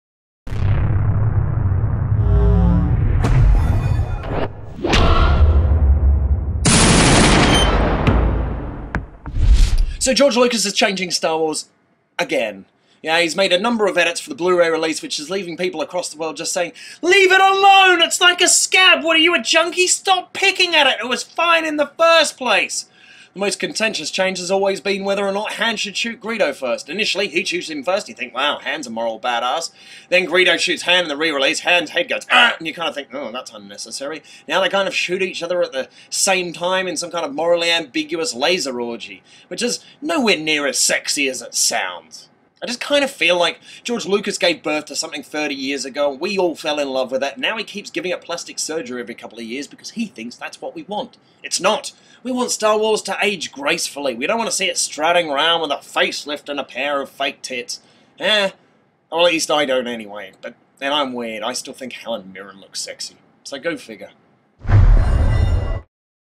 Puns 'n' Ammo: Hey Lucas, Leave Those Droids Alone!
Why Star Wars on Blu-ray is the equivalent of George Lucas picking a bloody scab.